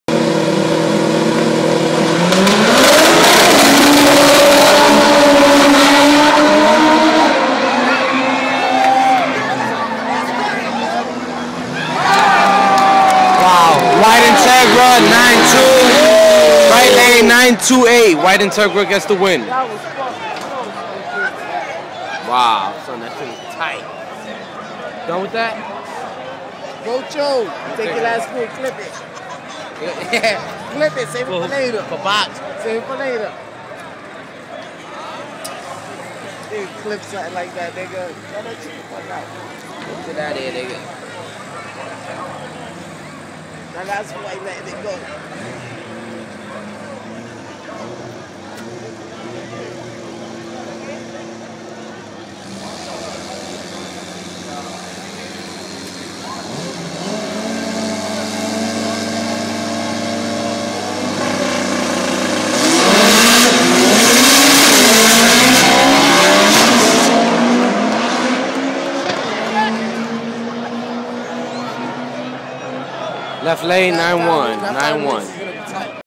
2009 compact fall nationals raceway park
Mobel sponcered Ecotec chevy loses to white integra in a close run.